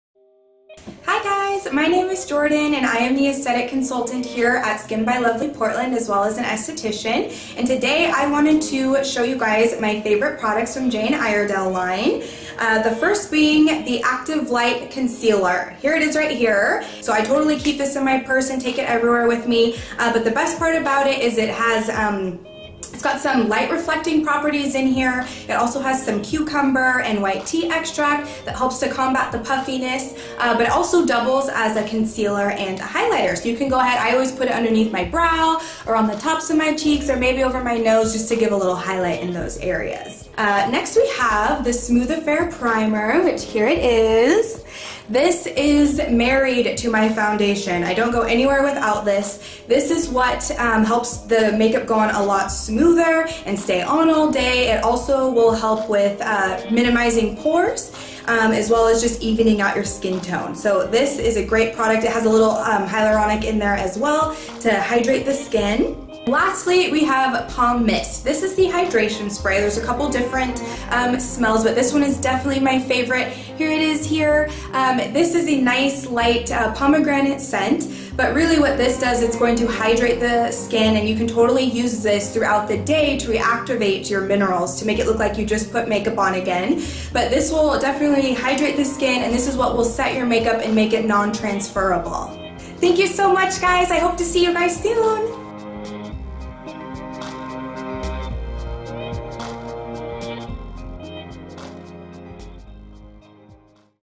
Skin by Lovely - 3 of our Favorite Jane Iredale Products
Skin by Lovely's Aesthetic Consultant, Jordan explains 3 of her favorite Jane Iredale products.